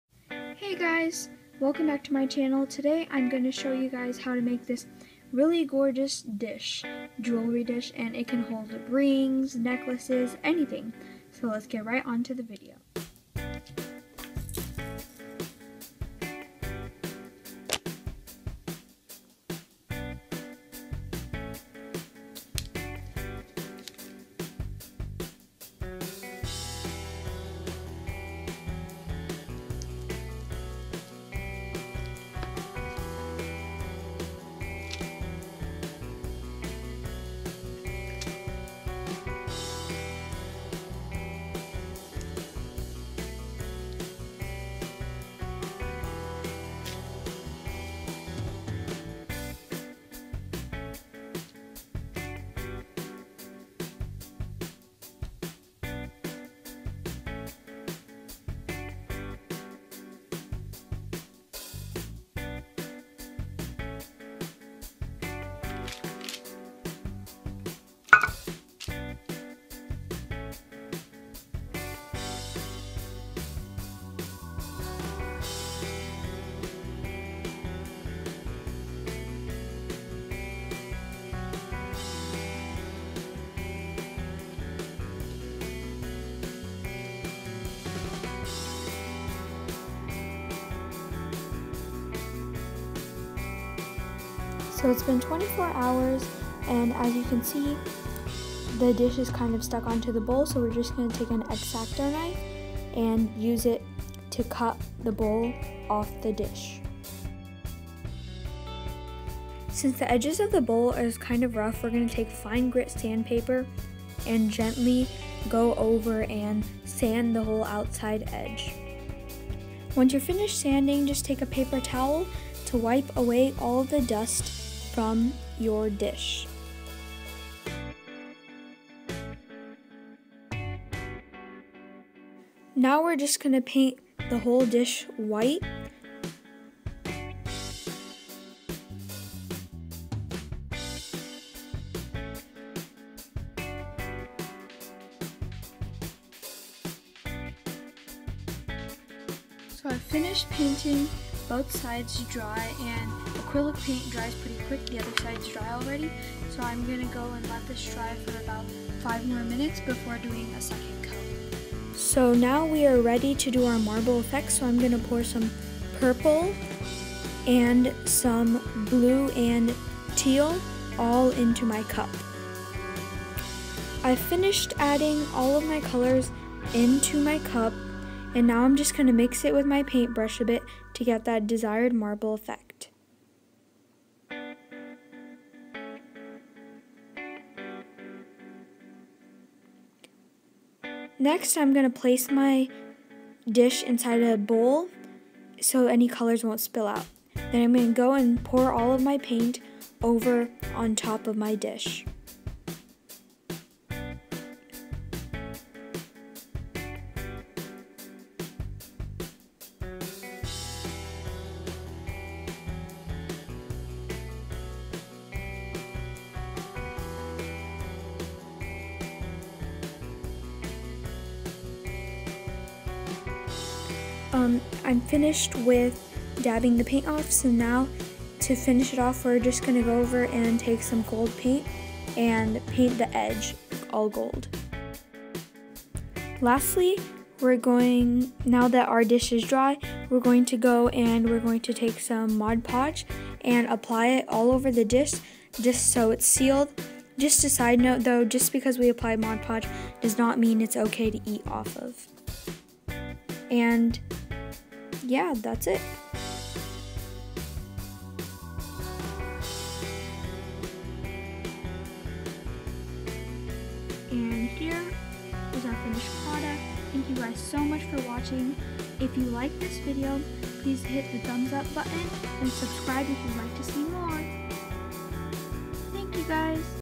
HOW TO: DIY Air-Dry Clay Jewelry Dish -- Easy and Fun Gift!
Hey guys! Today I will show you how to make this Easy DIY Air-Dry Clay Jewelry Dish. With Mother's Day coming up, it's the perfect gift!